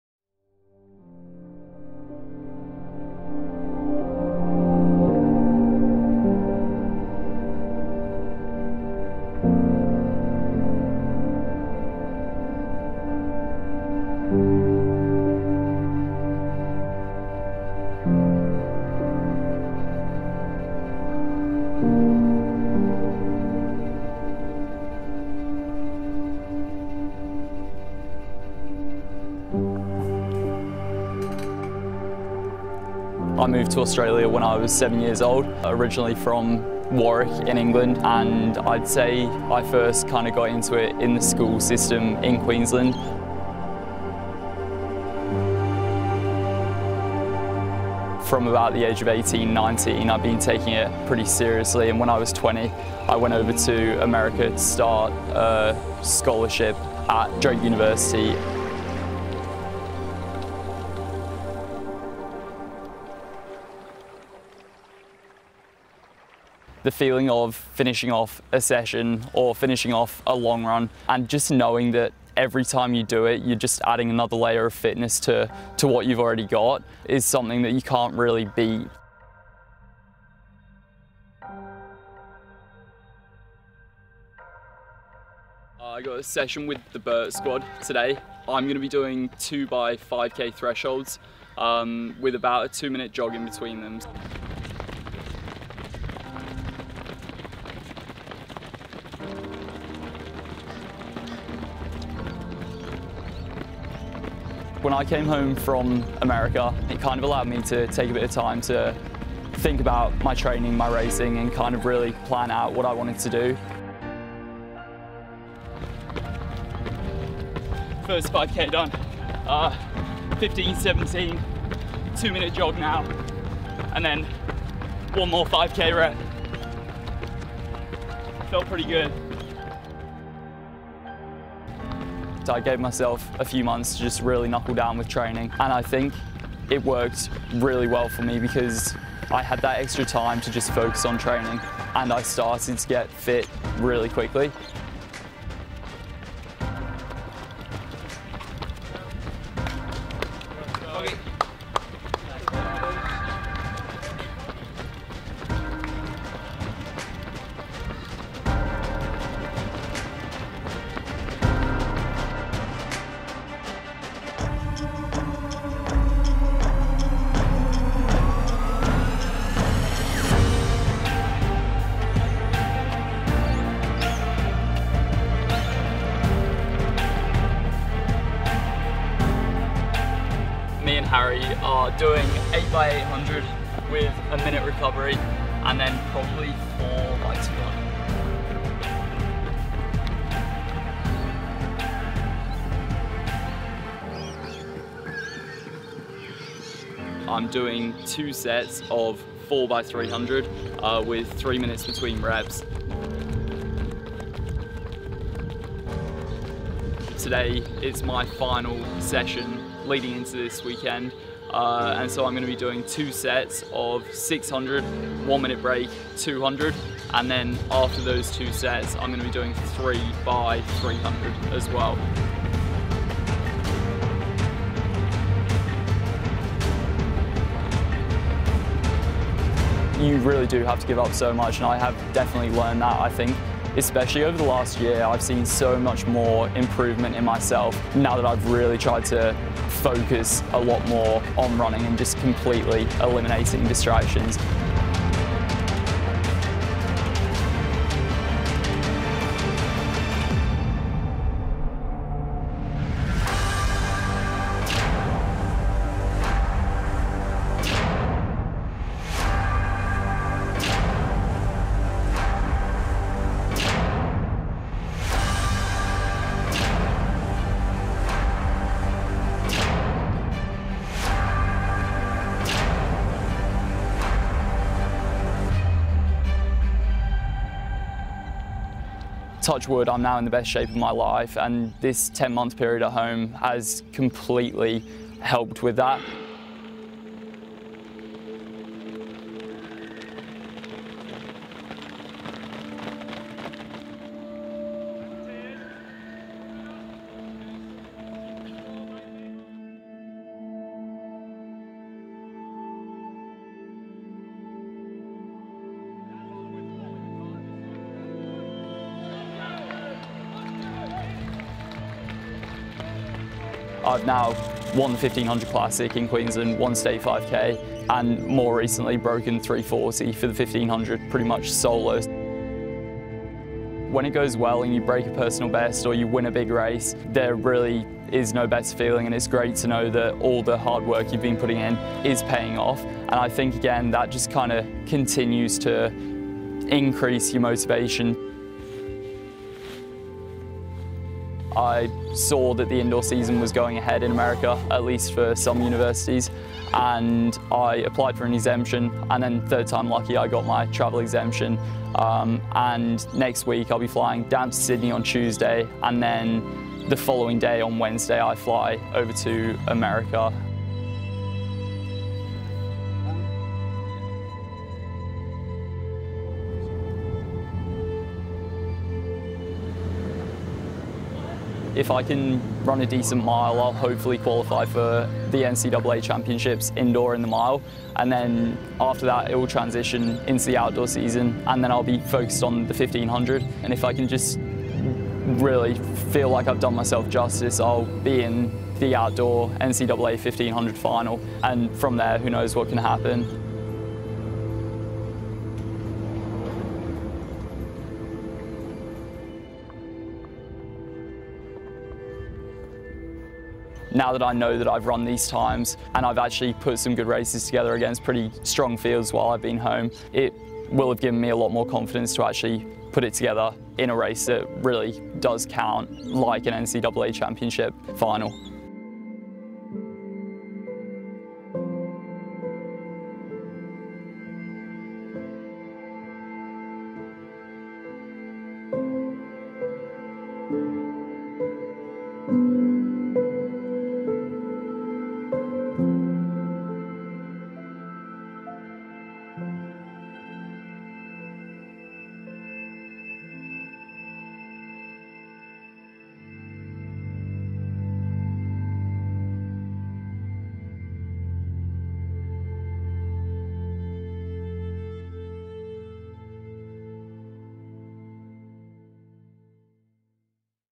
Adam Fogg | Documentary
A documentary on Australian middle distance runner, Adam Fogg. A young up and coming athlete, moments before he returns to America for his first NCAA championship race.